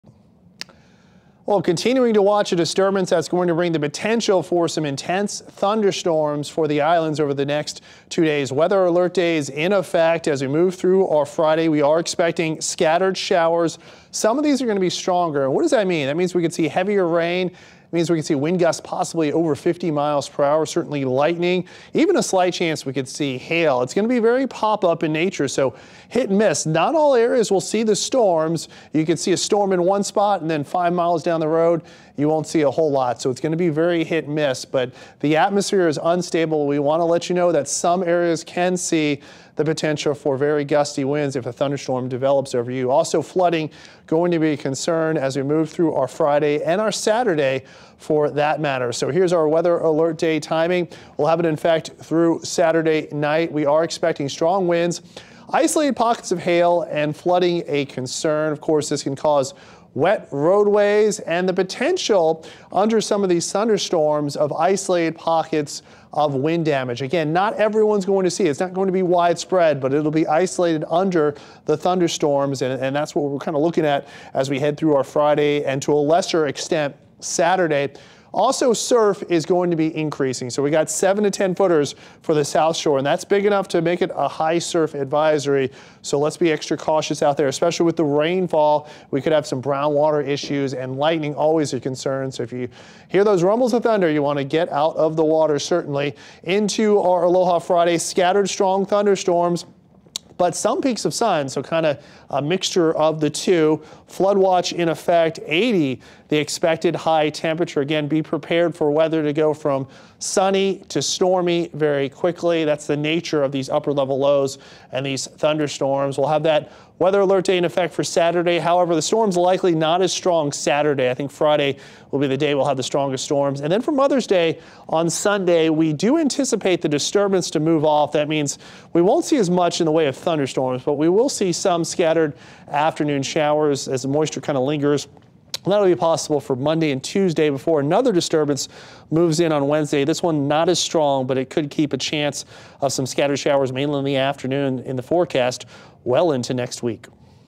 Aloha Friday Forecast - Strong thunderstorms possible Friday into Saturday
Weather Alert Days in effect Friday - Saturday as an upper low brings possible severe thunderstorms to the islands.